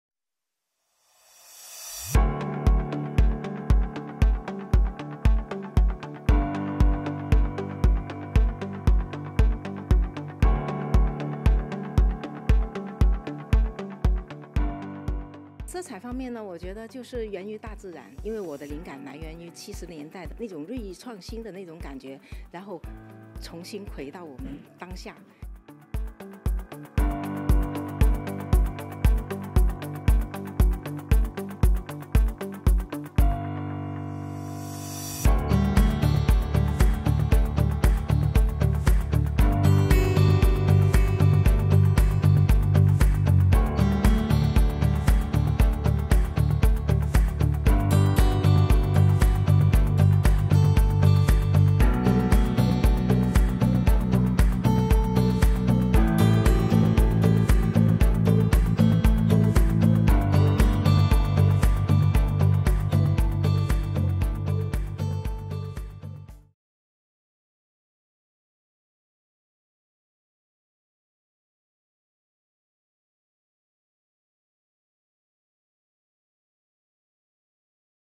Styles of Vintage, Modernity Feature Fashion Week
An eye-catching fashion show featuring styles of vintage elements integrated with modernity and traditional culture from south China was staged Thursday, presenting a visual feast to the audience at the ongoing China Fashion Week in Beijing.